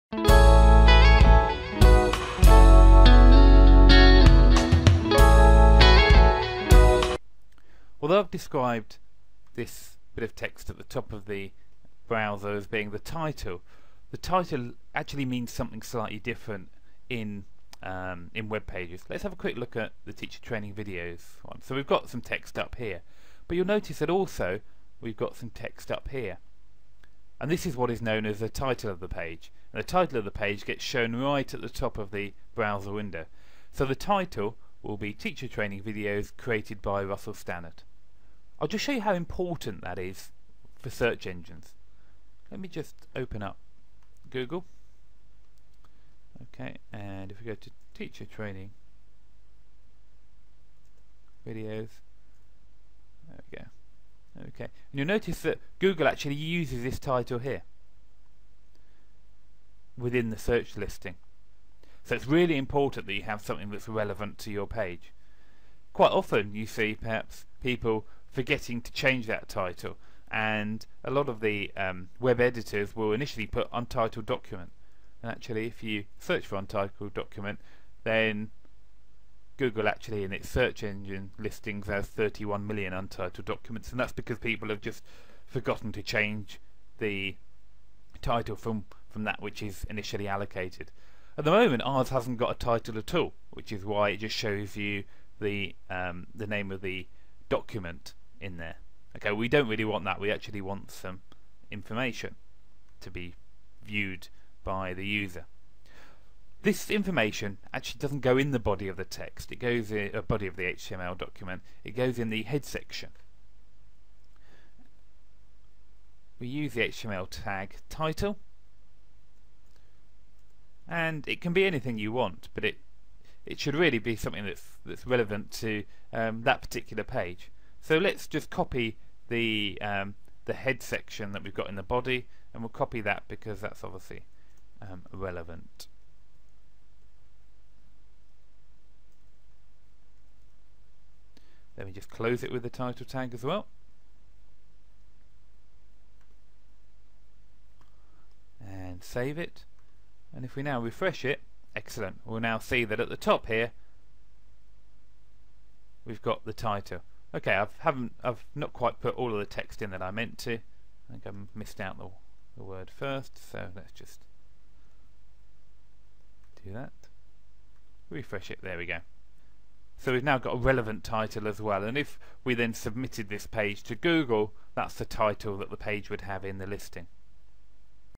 THE WEBPAGE TITLE
IN THIS LESSON YOU WILL LEARN HOW TO GIVE PAGE TITLE TO HTML PAGES.